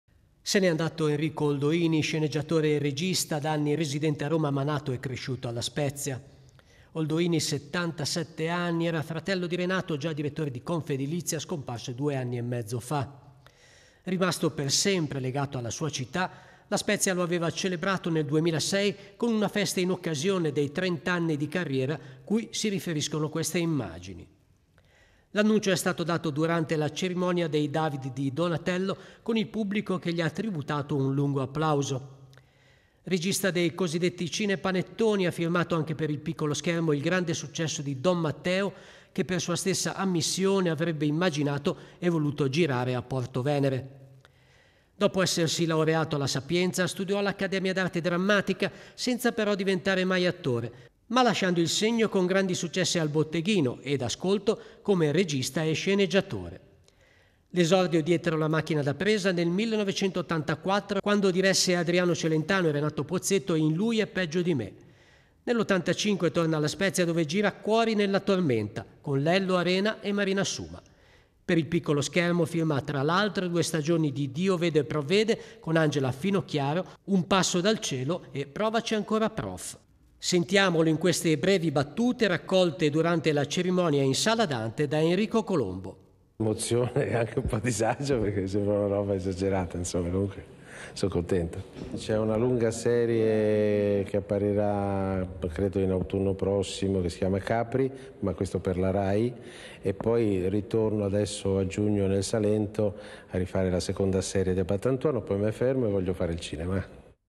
Morto Enrico Oldoini, regista e sceneggiatore spezzino 11-05-2023
È morto a Roma  lo sceneggiatore e regista Enrico Oldoini.
Aveva 77 anni appena compiuti: era nato a La Spezia il 4 maggio 1946 .
L'annuncio è stato dato durante la cerimonia dei David di Donatello da Carlo Conti e il pubblico gli ha tributato un lungo applauso.